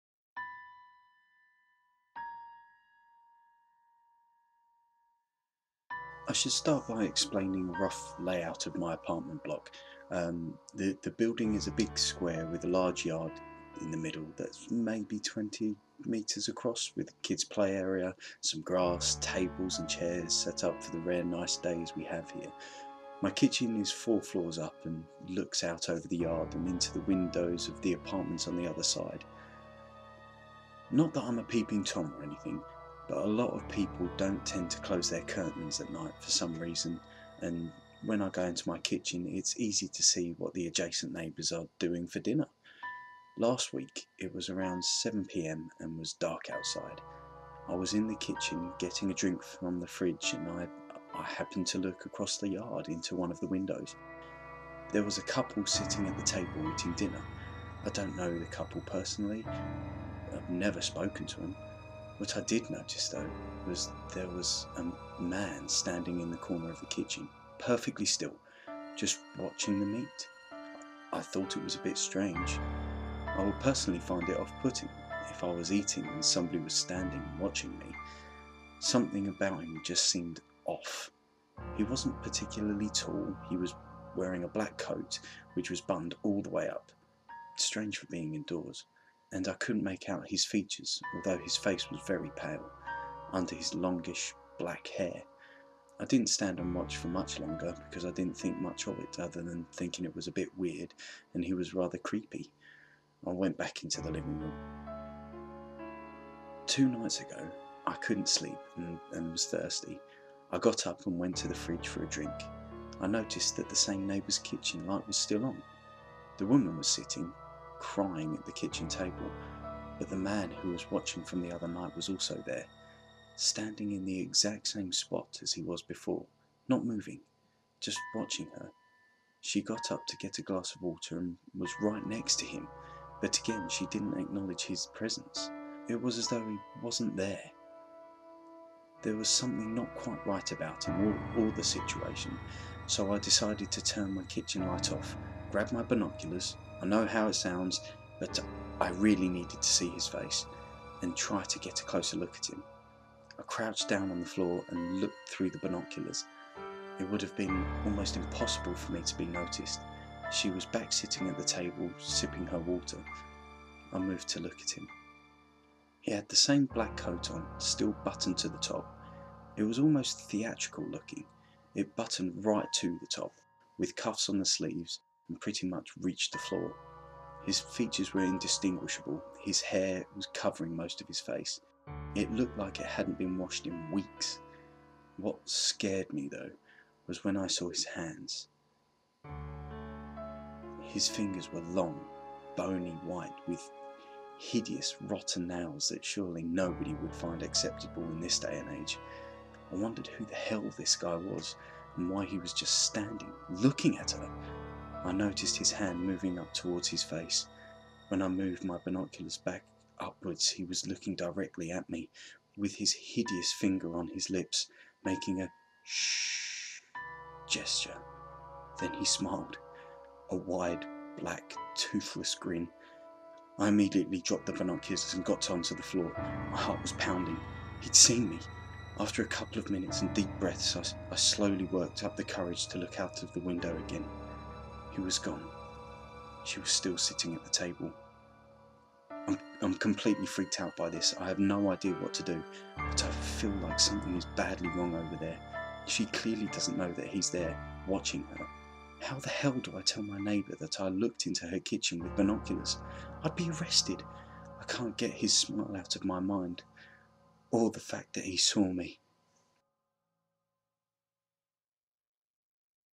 Creepypasta Let's Read: The Watcher
Welcome to my Creepypasta Let's Read Series! Stories narrated by Sean Antony from the  reddit/NOSLEEP page.

Transcription:

I should start by explaining a rough layout of my apartment block. The building is a big square with a large yard in the middle that's maybe 20 metres across with a kids play area, some grass, tables and chairs set up for the rare nice days we have here. My apartment is 4 floors up and the kitchen looks out over the yard and into the windows of the apartments on the other side.
Not that I'm a peeping tom or anything, but a lot of people don't tend to close their curtains at night for some reason and when I go into my kitchen, it's easy to see what the adjacent neighbours are doing for dinner.
Last week, it was around 7pm and was dark outside. I was in the kitchen getting a drink from the fridge and I happened to look across the yard into one of the windows. There was a couple sitting at the table eating dinner. I don't know the couple personally, have never spoken to them. What I did notice though was that there was a man standing in the corner of the kitchen, perfectly still; just watching them eat. I thought it was a bit strange, I would personally find it off-putting if I was eating and somebody was standing watching me. Something about him just seemed "off". He wasn't particularly tall, he was wearing a black coat, which was buttoned all the way up, (strange for being indoors) and I couldn't make out his features although his face was very pale, under his longish, black hair. I didn't stand and watch for much longer because I didn't think much of it other than thinking it was a bit weird and he was rather creepy. I went back to the living room.
2 nights ago, I couldn't sleep and was thirsty. I got up and went to the fridge for a drink. I noticed that the same neighbour's kitchen light was still on. The woman was sitting, crying at the kitchen table, but the man who was watching from the other night was also there. Standing in the exact same spot as he was before, not moving. Just watching her. She got up to get a glass of water and was right next to him, but again she didn't acknowledge his presence. It were as though he wasn't there. There was something not quite right about him or the situation so I decided to turn my kitchen light off, grab my binoculars, (I know how it sounds but I really needed to see his face) and try to get look closer at him. I crouched down on the floor and looked through the binoculars; it would have been almost impossible for me to be noticed. She was back sitting at the table, sipping her water. I moved to look at him.
He had the same black coat on, still buttoned to the top. It was almost theatrical looking, it buttoned right to the top, with cuffs on the sleeves and pretty much reached the floor. His features were indistinguishable; his hair was covering most of his face, it looked like it hadn't been washed in weeks. What scared me though was when I saw his hands. His fingers were longer than I would normally expect, bony white, almost skeletal with long, rotten nails that surely nobody would find acceptable in this day and age. I wondered who the hell this guy was and why he was just standing looking at her! I noticed his hand moving up towards his face. When I moved my binoculars back upwards he was looking directly at me with his hideous finger on his lips making a "shhh" gesture. Then he smiled. A wide, black, toothless grin. I immediately dropped the binoculars and got on the floor, heart pounding. He'd seen me! After a couple of minutes and deep breaths, I slowly worked up the courage to look out of the window again. He was gone. She was still sitting at the table.
I'm completely freaked out by this, I have no idea what to do, but I feel like something is badly wrong over there. She clearly doesn't know that he's there, watching her. How the hell do I tell my neighbour that I looked into her kitchen with binoculars? I'd be arrested. I cannot get his smile out of my mind or the fact that he saw me.